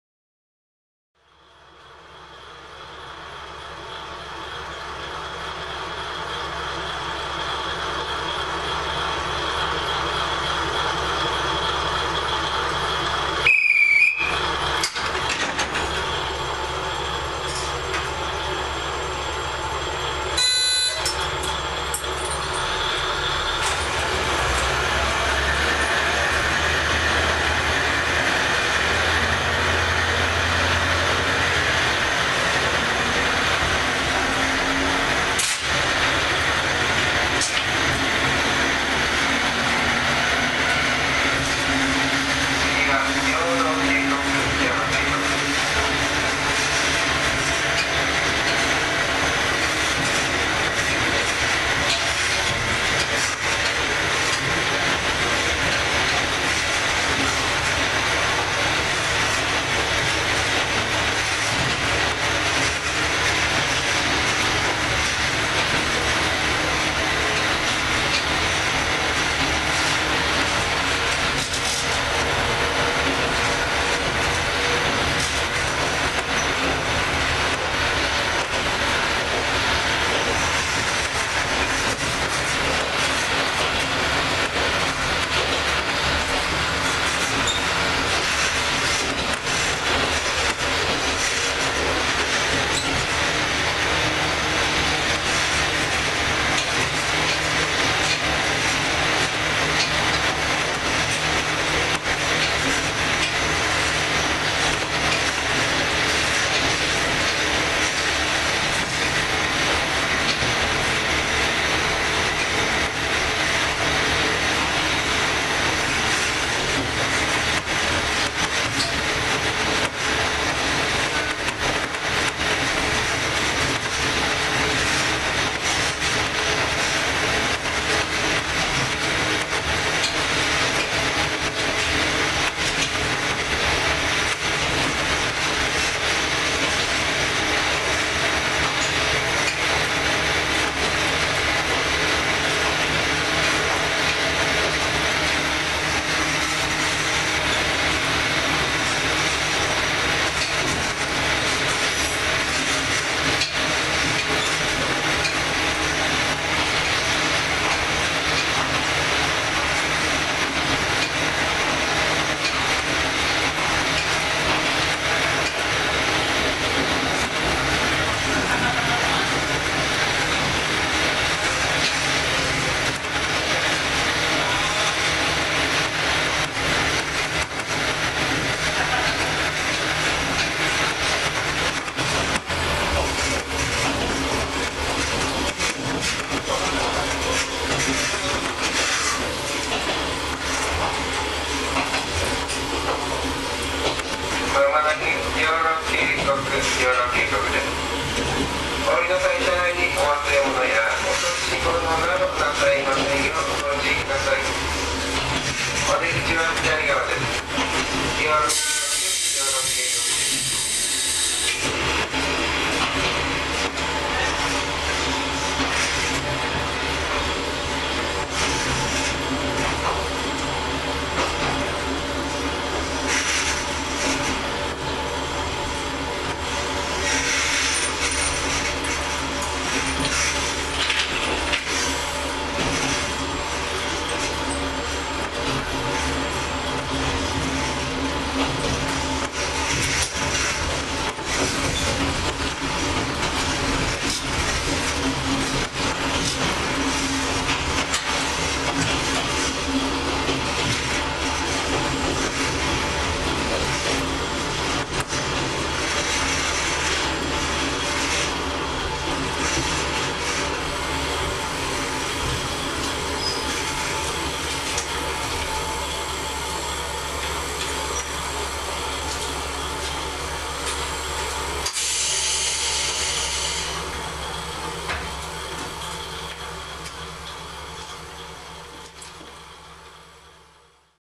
小湊鐵道キハ200形 走行音 Kominato Railway Type DC 200 sound
小湊鐵道キハ200形の走行音です。
機関：DMH17C形
変速機：TC-2形
録音区間：小湊鉄道線 上総大久保～養老渓谷
※ご視聴の際は、音量にご注意下さい※
【小湊鐵道キハ200形について（Wikipedia）】

Kominato Railway
Type DC 200  sound
Kominato line Kazusa-Ōkubo→Yōrō-Keikoku
This video is sound only.
◇Please note the sound volume when you watch this video◇